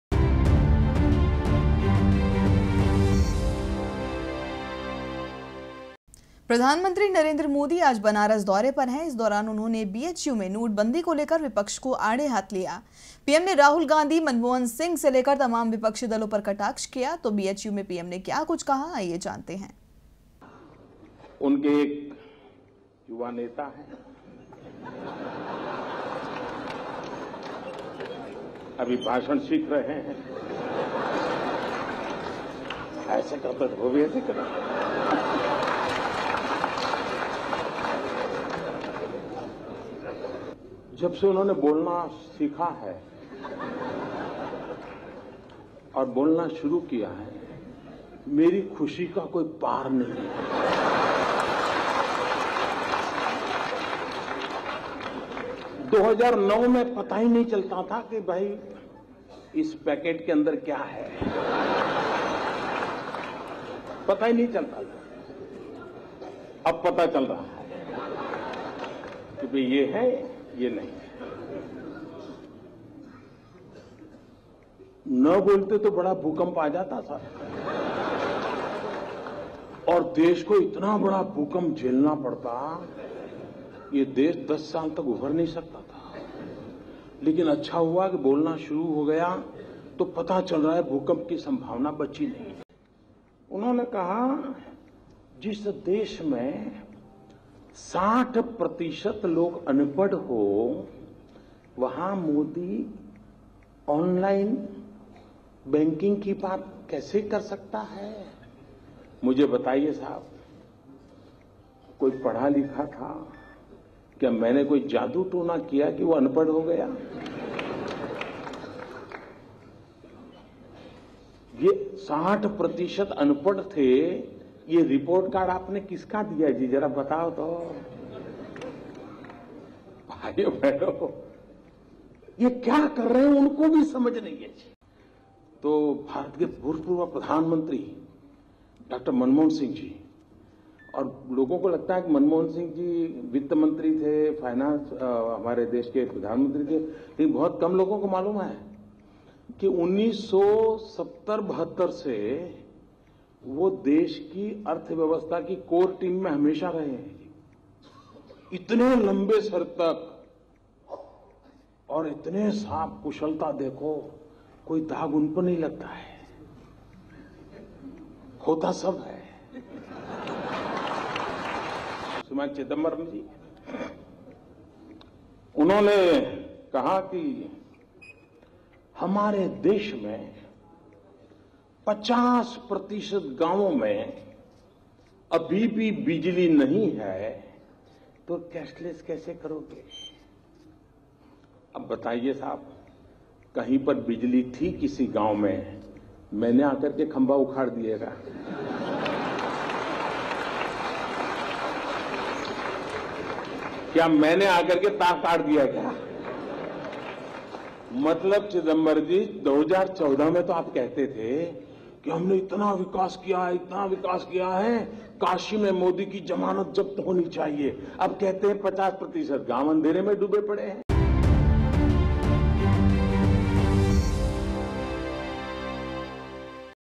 DB LIVE | 22 DEC 2016 | Narendra Modi rally in Varanasi
प्रधानमंत्री नरेंद्र मोदी आज बनारस दौरे पर हैं. इस दौरान उन्होंने बीएचयू में नोटबंदी को लेकर विपक्ष को आड़े हाथ लिया. पीएम ने राहुल गांधी, मनमोहन सिंह से लेकर तमाम विपक्षी दलों पर कटाक्ष किया..तो बीएचयू में पीएम ने क्या कुछ कहा आईए जानते हैं..